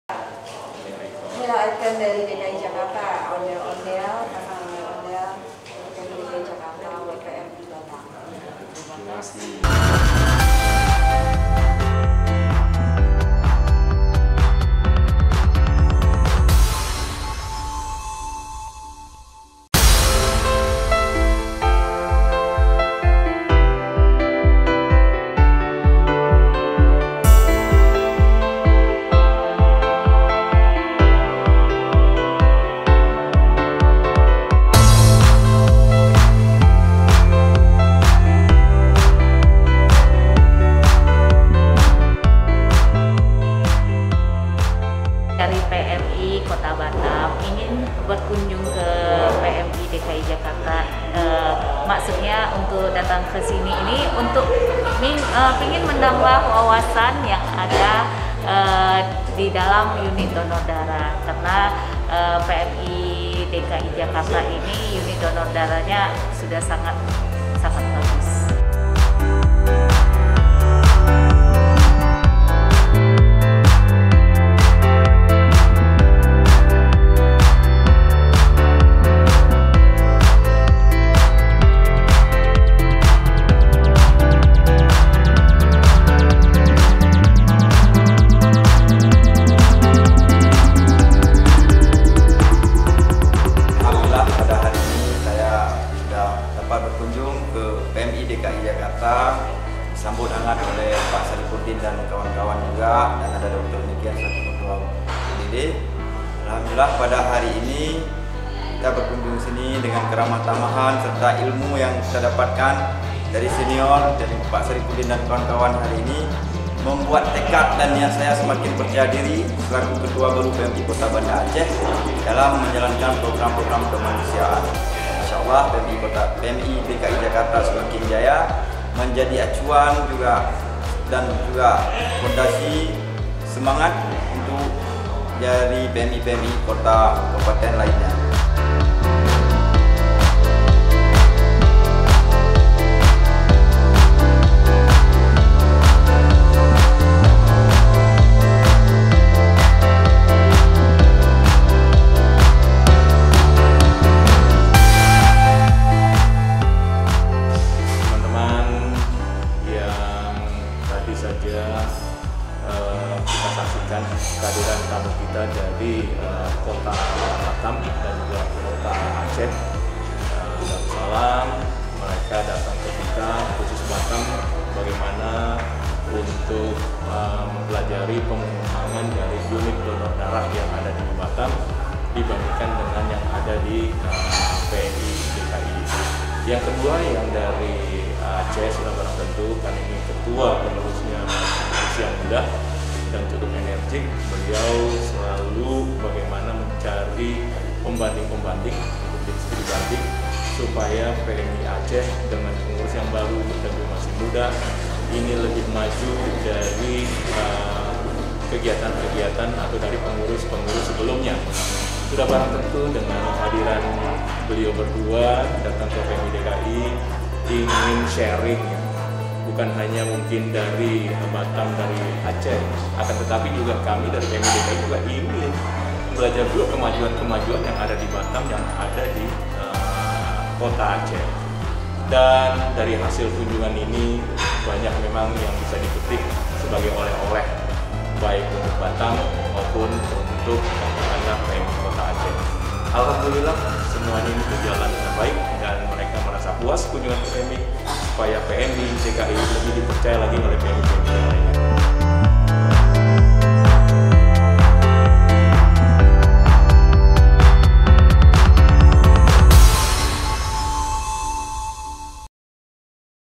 Berkunjung, PMI Aceh & Batam Pelajari Tata Kelola UDD PMI DKI Jakarta
PMI DKI Jakarta menerima kunjungan dari PMI Kota Batam dan Kota Banda Aceh. Kedua pengurus PMI daerah tersebut diterima Pengurus PMI DKI Jakarta di Gedung PMI DKI Jakarta, Jalan Kramat Raya 47, Senen, Jakarta Pusat, Kamis (16/12/2021).

Dalam kunjungan, PMI Kota Batam dan PMI Kota Banda Aceh melakukan sharing dan informasi terkait dengan tata kelola dan mekanisme operasional Unit Donor Darah (UDD) di PMI DKI Jakarta.